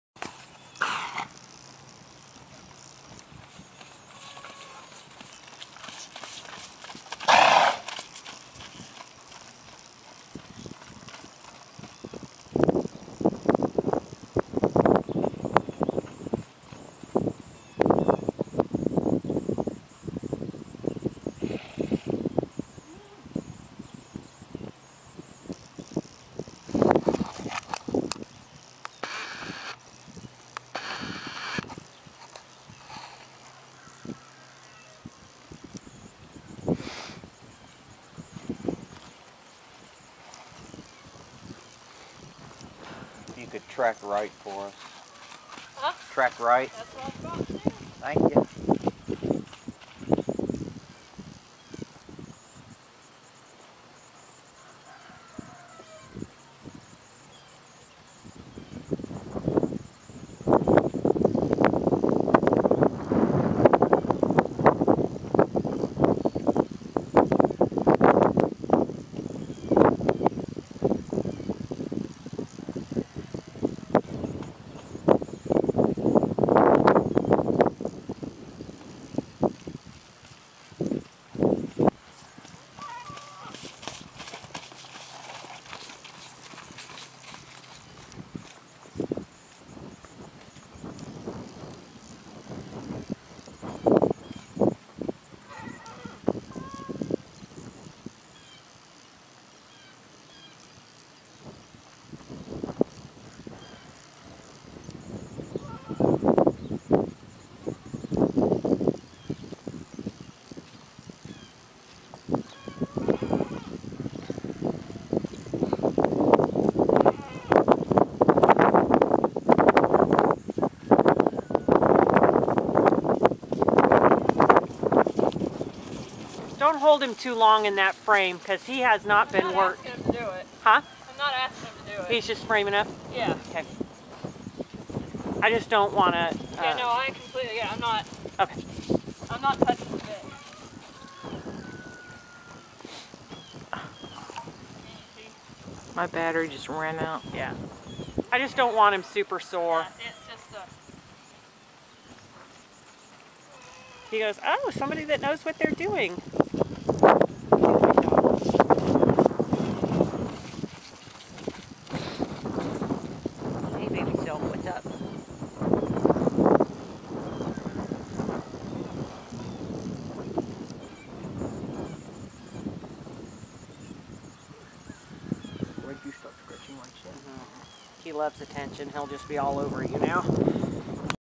Shadow PPE
Steve, Caleb, found this from March 2014 - shows NO traveling on the centerline or hind interference.